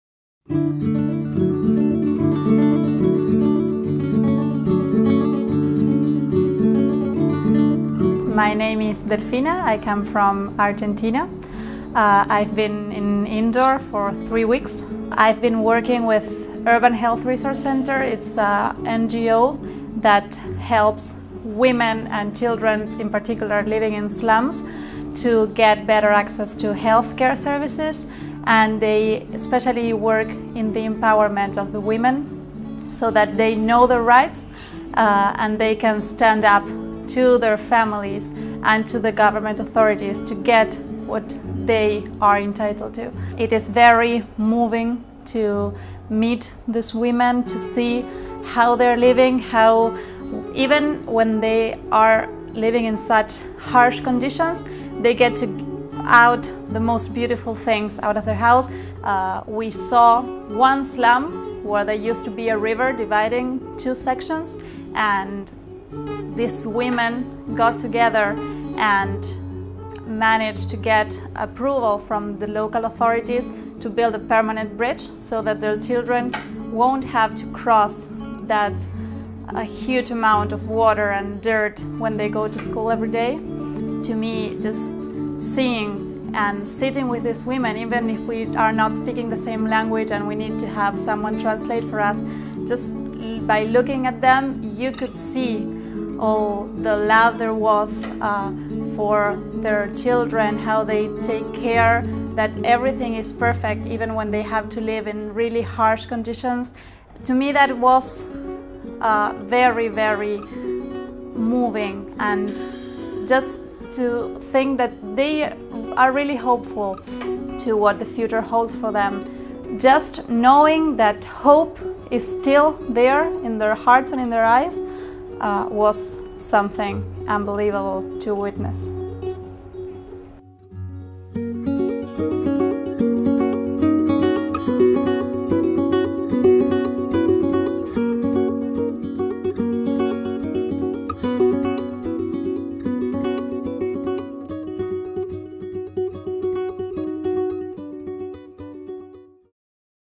IBMer Delfina Daglio shares her Corpoarte Service Corps (CSC) Experience at Indore - India
Delfina Daglio from IBM Argentina and is part of Team India 17 India of the Corporate Service Corps. In this video, she speaks about her experience with the Urban Health Research Center, which addresses health, nutrition and well-being of the disadvantaged urban dwellers.

She reflects on her experience of women living in the slums and their attitude and gumption in overcoming tremendous adversity and the perseverance of hope.